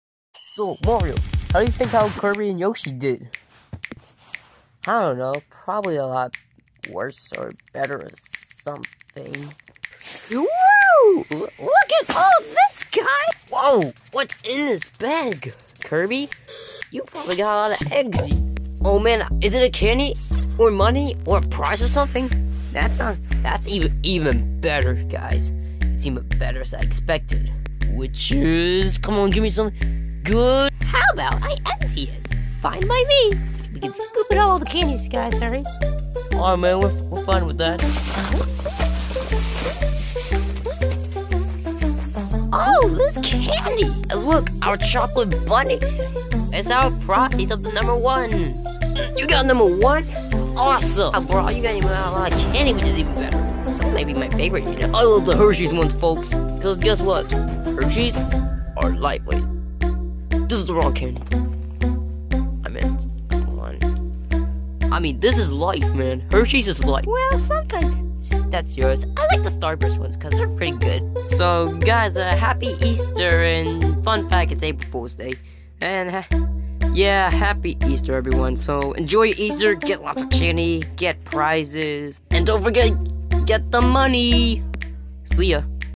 Kirby and Yoshi's candies after egg hunting
Kirby and Yoshi we're doing the Easter egg hunt